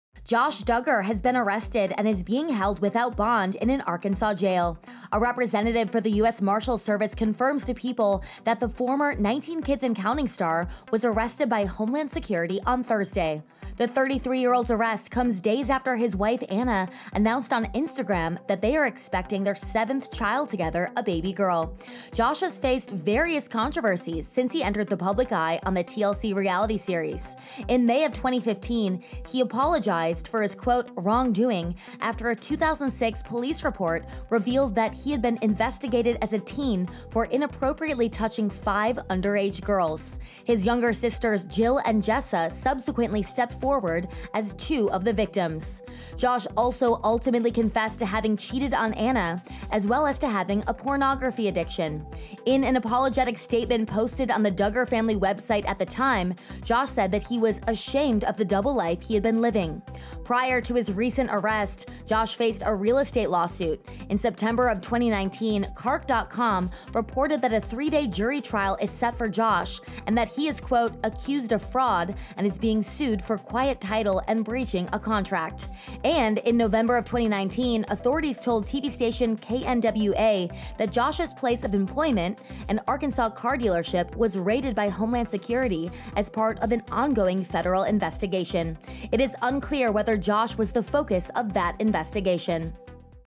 Josh Duggar Arrested, Being Held in Jail in Arkansas | PEOPLE
Online jail records do not indicate what charges he was arrested on when he was booked.

Josh Duggar has been arrested and is being held without bond in an Arkansas jail.

The former 19 Kids and Counting star was arrested by Homeland Security on Thursday, a representative for the U.S. Marshals Service confirms to PEOPLE. Online jail records do not indicate what charges he was arrested on when he was booked.